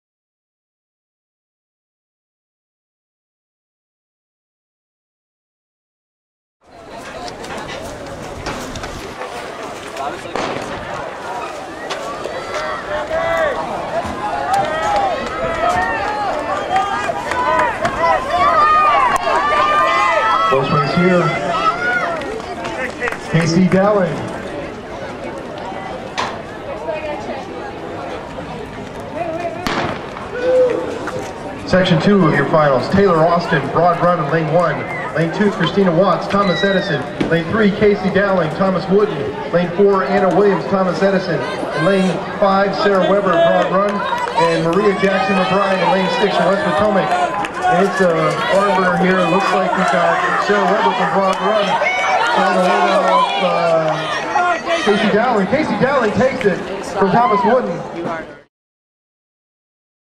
Wootton at 2011 XBC - Girls 100 and 200 Finals
XBC Track Experience; Chantilly HS; April 30, 2011; times are approximate